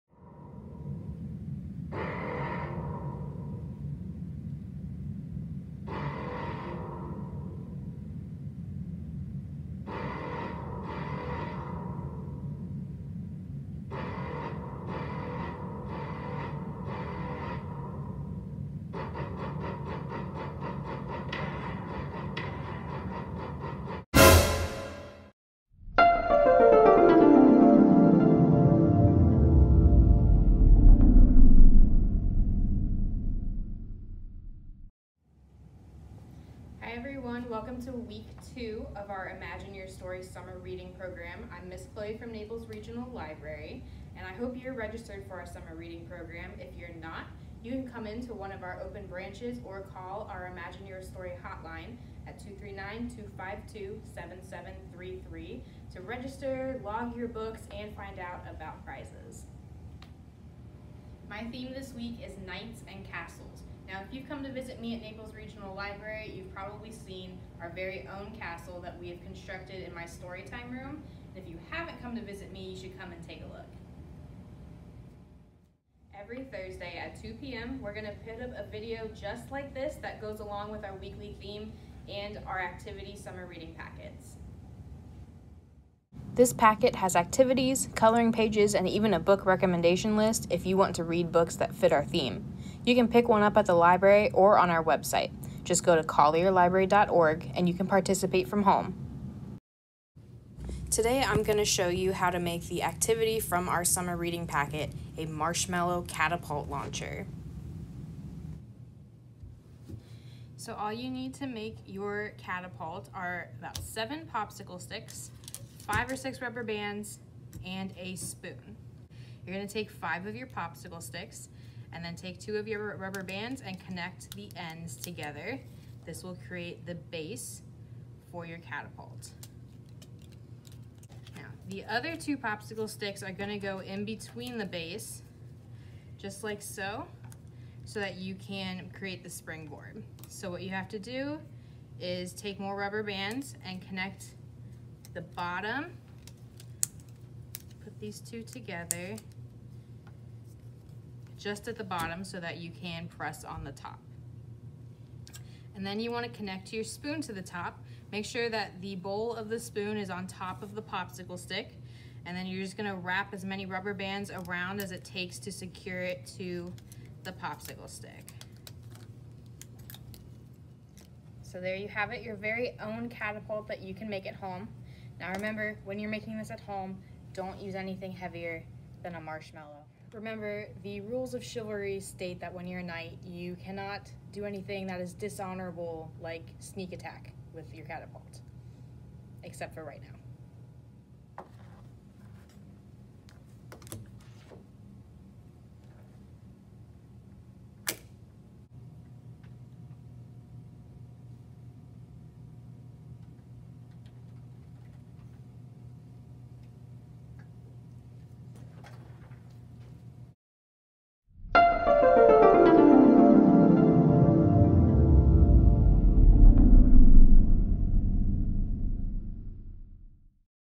Imagine Your Story 2020: Episode 7 - Knights and Castles - Collier County Public Library
Imagine Your Story Summer Reading Program 2020. Join Chloe as she shows you an easy way to make your own catapult!